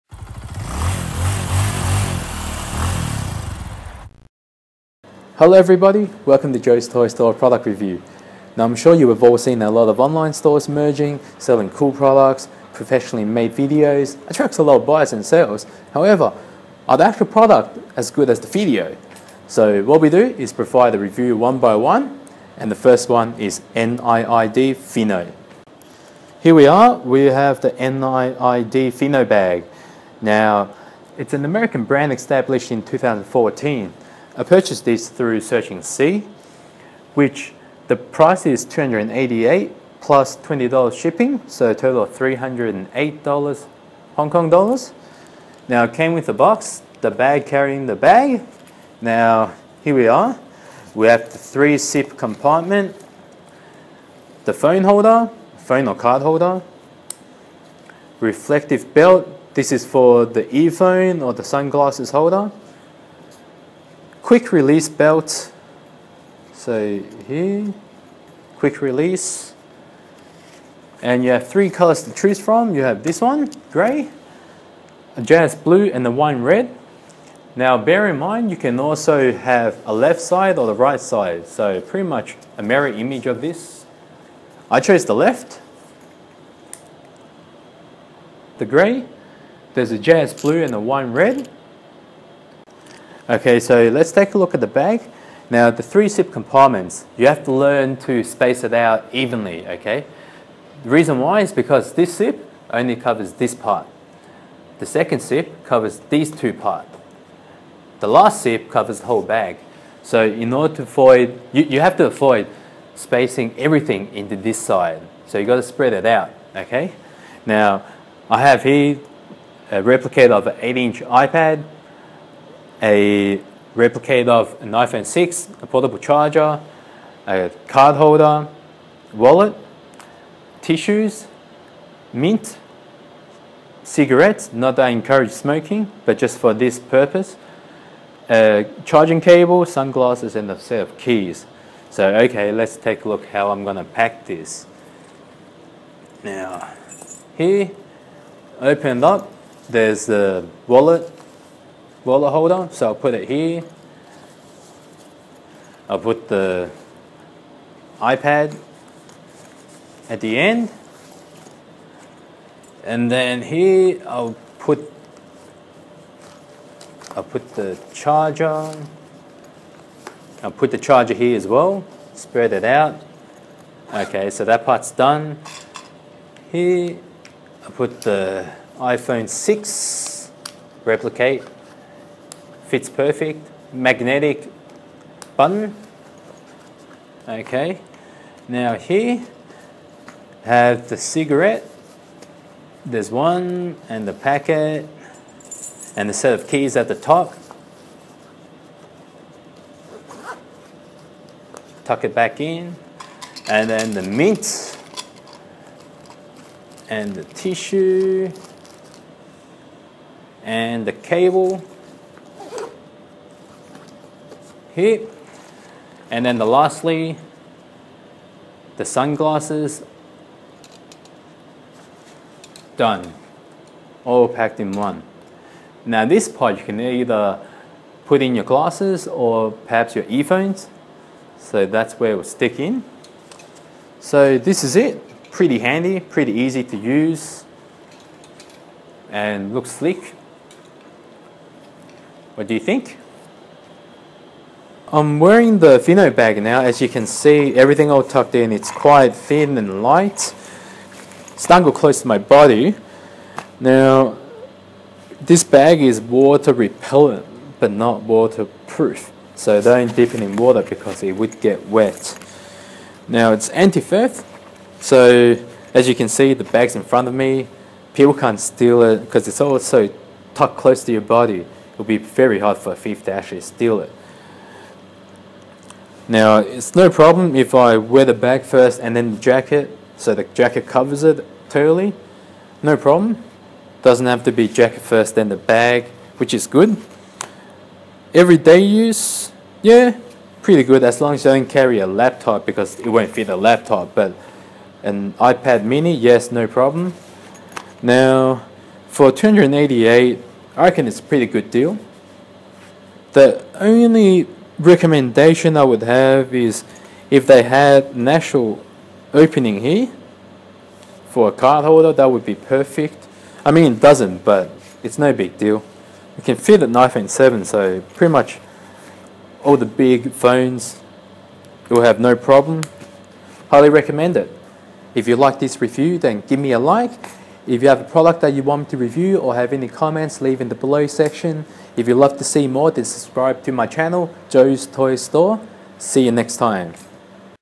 NIID FINO bag review (ENG)
Great little day bag!
Appearance: 8, very unique
Comfortable: 7 with iPad Mini / 10 without
Usability: 9, only if they had a dedicated card slot then it would be a 10.